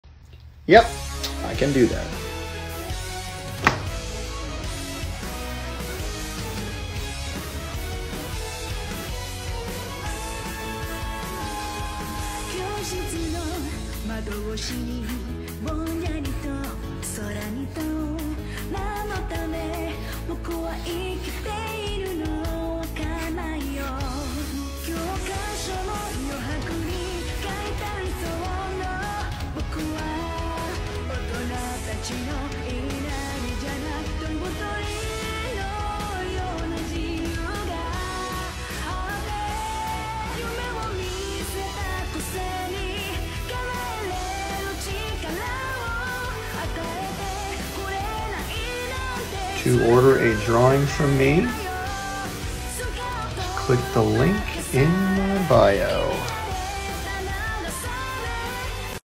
Drawing ZERO TWO from DARLING in the FRANXX!
Who's is YOUR fave anime' character? And what other anime' characters should I draw?